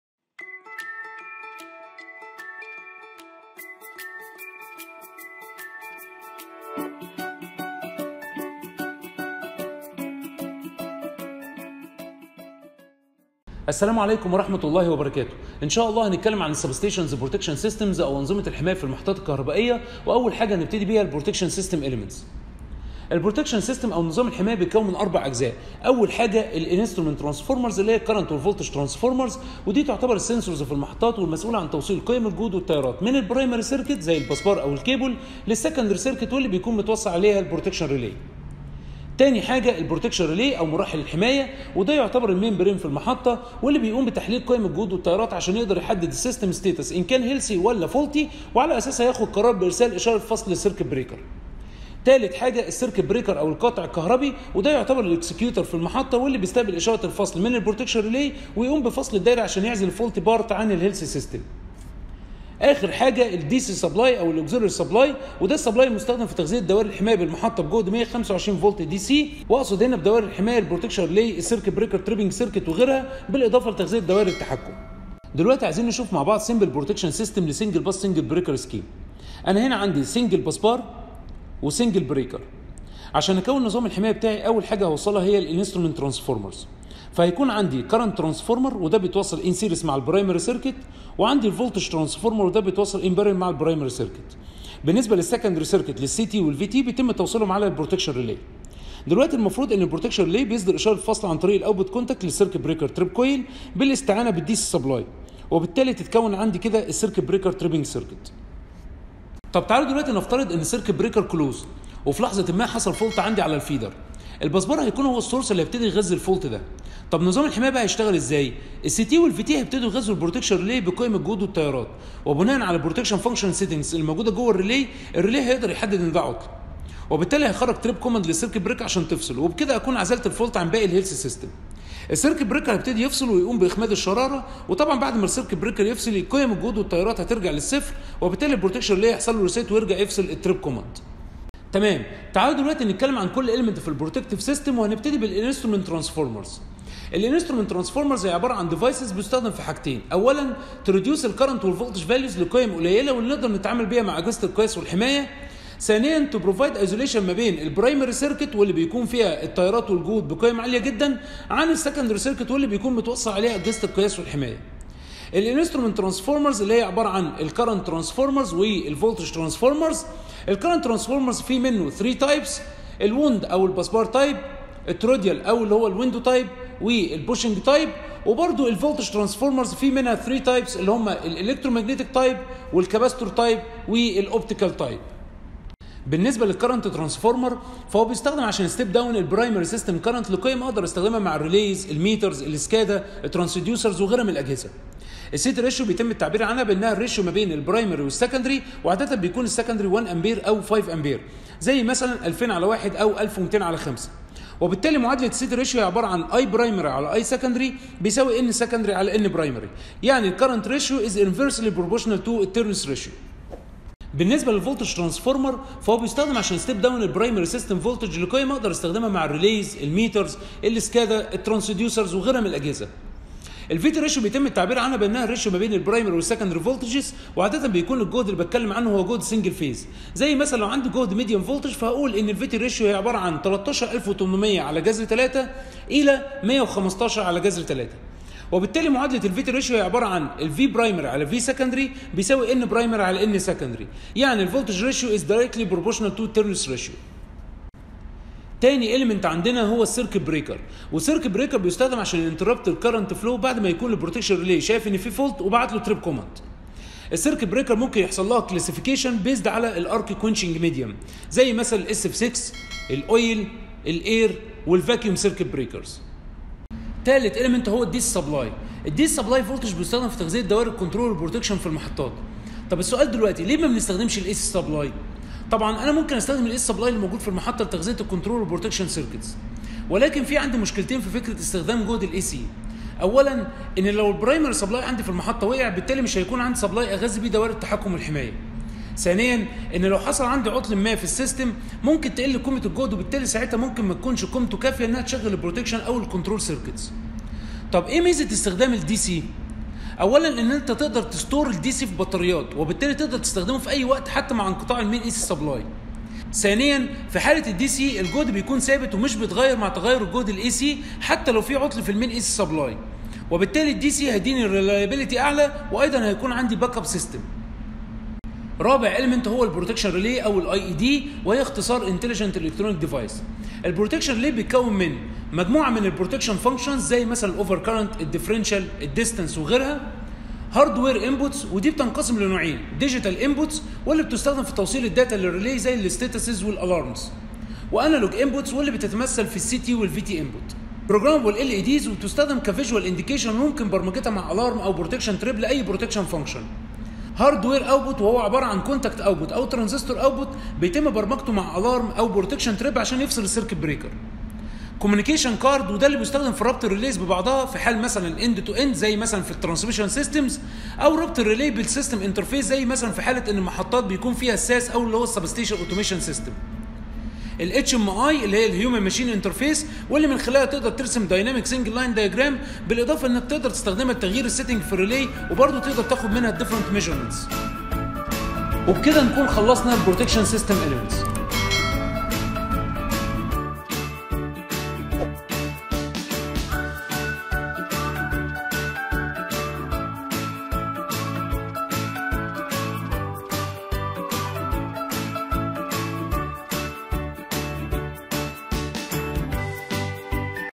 Substations Protection Systems "Protection System Elements" *Lecture 01*
An Electrical Grid is an interconnected network for delivering electricity from suppliers to consumers. It consists of generating stations that produce electrical power,  high-voltage transmission lines that carry power from distant sources to demand centers, and distribution lines that connect individual customers.
Power system protection is a branch of electrical power engineering that deals with the protection of electrical power systems from faults through the isolation of faulted parts from the rest of the electrical network.
Power system protection is the art and science of detecting problems with power system components and isolating these components.

The main function of protection is to selectively and rapidly detect and disconnect a fault on the protected circuit.

Protective System Elements
Instrument Transformers (C.T.& V.T.)
DC Supply.
Fuses.
Circuit Breaker .
Protection Relays.

Components of Power System Need to be Protected
Generator
Transformers
Bus-bars.
Over Head Transmission Lines
Outgoing Feeders
Capacitor Banks
Shunt Reactors
SVC